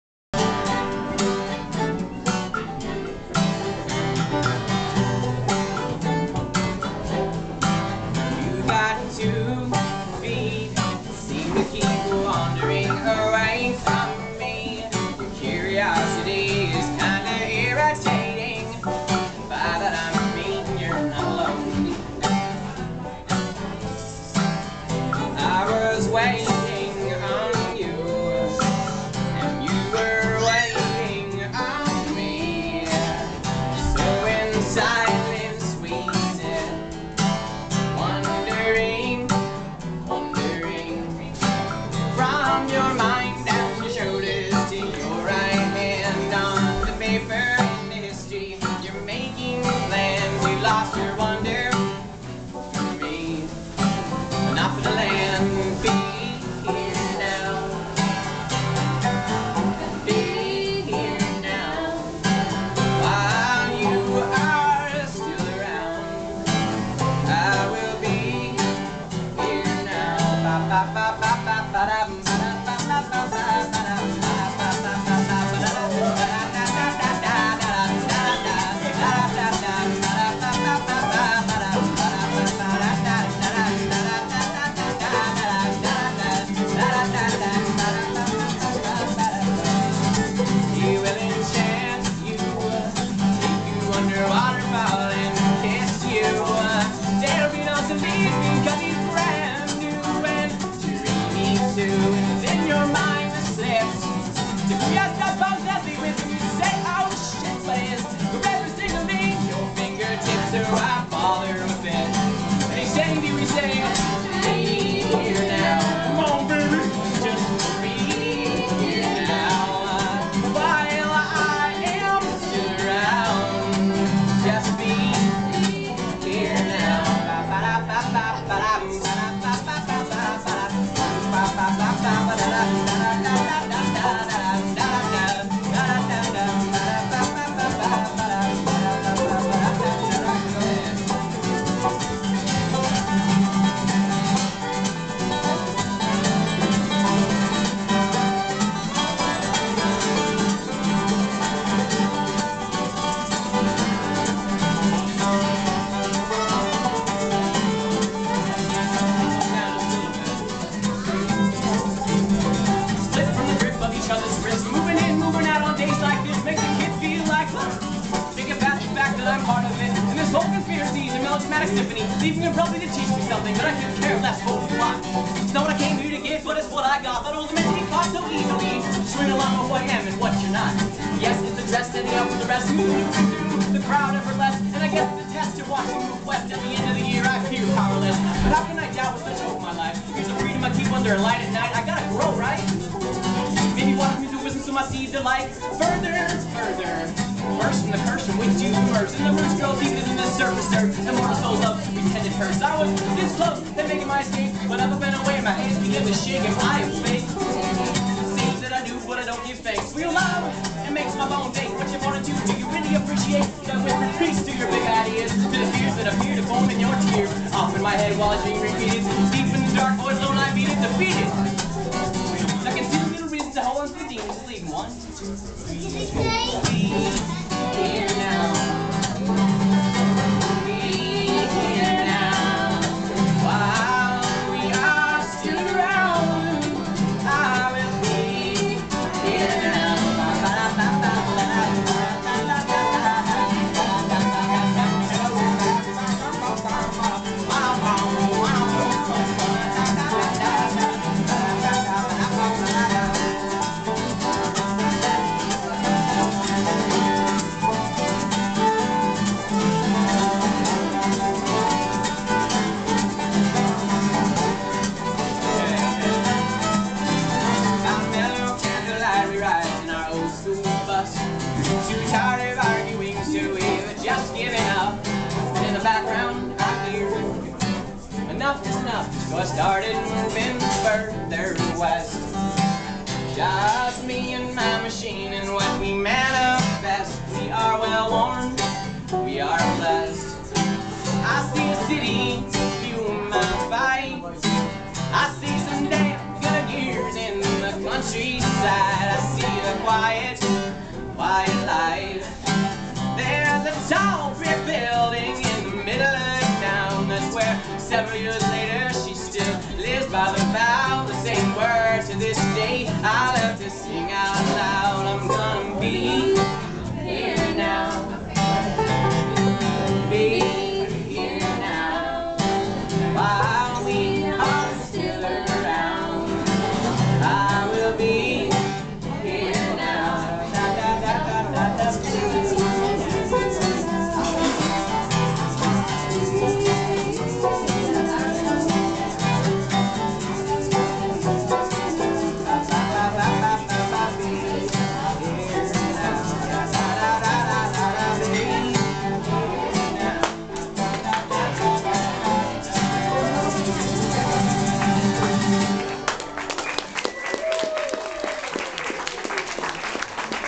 Nahko Kope Kope
Nahko, Adele, Jon and Mary's first performance together at Kope Kope's (cafe) in HILO. April. 17 2008